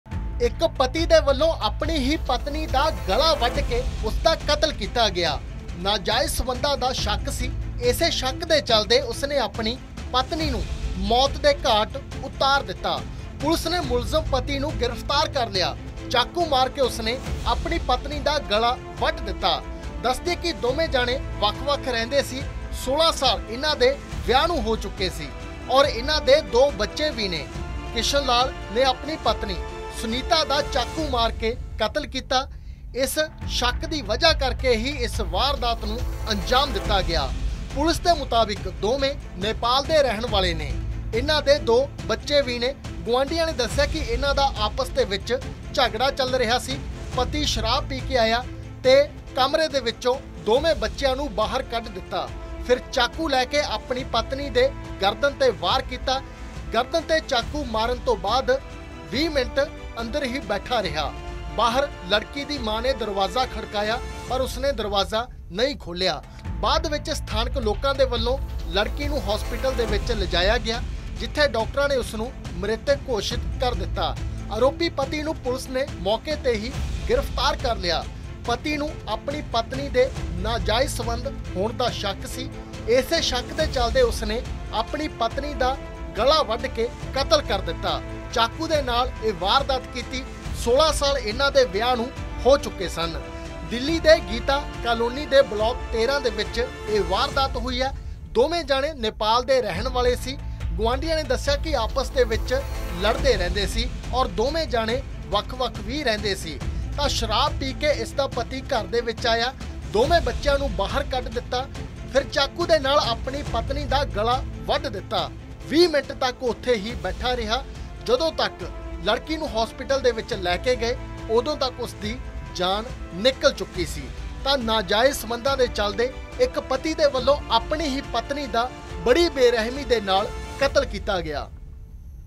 Delhi Murder News | ਪਤੀ ਨੇ ਵੱਢ 'ਤਾ ਪਤਨੀ ਦਾ ਗਲ..! ਨਾਜਾਇਜ਼ ਸੰਬੰਧ ਹੋਣ ਦਾ ਸੀ ਸ਼ੱਕ | Extra Marital Affairs
ਪਤੀ ਨੇ ਵੱਢ 'ਤਾ ਪਤਨੀ ਦਾ ਗਲ..! ਨਾਜਾਇਜ਼ ਸੰਬੰਧ ਹੋਣ ਦਾ ਸੀ ਸ਼ੱਕ, 2 ਬੱਚਿਆਂ ਨੂੰ ਘਰੋਂ ਕੱਢ ਕੇ ਕੀਤੀ ਵਾਰਦਾਤ